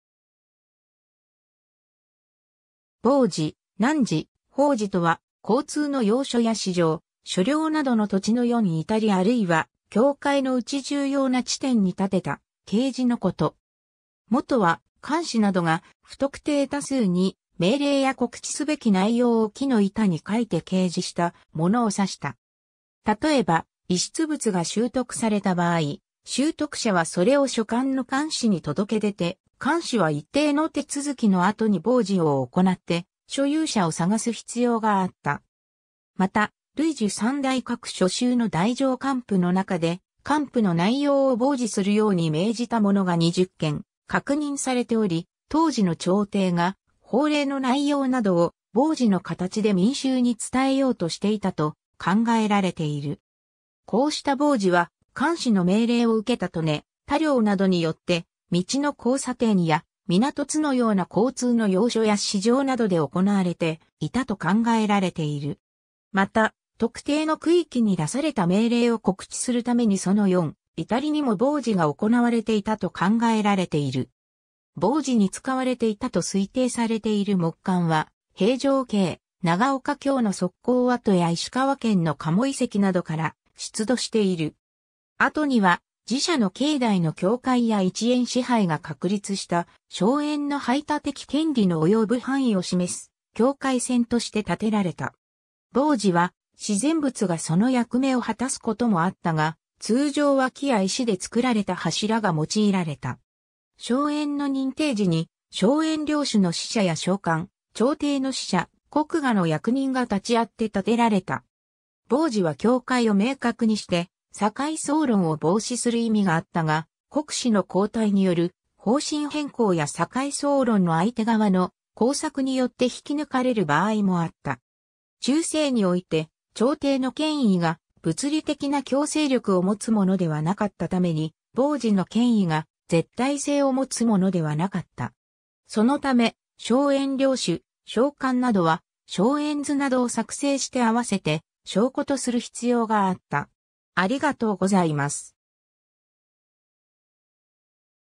牓示
牓示、牓爾、榜示（ほうじ、ぼうじ）とは、交通の要所や市場、所領などの土地の四至あるいは境界のうち重要な地点に立てた掲示のこと。
 元は、官司などが、不特定多数に命令や告知すべき内容（牓）を木の板に書いて掲示した物を指した。
例えば、遺失物が拾得された場合、拾得者はそれを所管の官司に届け出て、官司は一定の手続の後に牓示を行って所有者を探す必要があった（捕亡令官私奴婢条・得闌遺物条、雑令公私材木条）。
また、『類聚三代格』所収の太政官符の中で、官符の内容を牓示するように命じたものが20件確認されており、当時の朝廷が法令の内容などを牓示の形で民衆に伝えようとしていたと考えられている。
こうした牓示は官司の命令を受けた刀禰・田領などによって、道の交差点や港津のような交通の要所や市場などで行われていたと考えられている。
また、特定の区域（寺社の境内や山野など）に出された命令を告知するためにその四至にも牓示が行われていたと考えられている。
牓示に使われていたと推定されている木簡は平城京・長岡京の側溝跡や石川県の加茂遺跡などから出土している。
 後には、寺社の境内の境界や一円支配が確立した荘園の排他的権利の及ぶ範囲を示す境界線として立てられた。
牓示は自然物（岩や立木など）がその役目を果たすこともあったが、通常は木や石で作られた柱が用いられた。
荘園の認定時に荘園領主の使者や荘官、朝廷の使者、国衙の役人が立ちあって立てられた。
牓示は境界を明確にして境相論を防止する意味があったが、国司の交替による方針変更や境相論の相手側の工作によって引き抜かれる場合もあった。
中世において朝廷の権威が物理的な強制力を持つものではなかったために牓示の権威が絶対性を持つものではなかった。
そのため、荘園領主・荘官などは荘園図などを作成して合わせて証拠とする必要があった。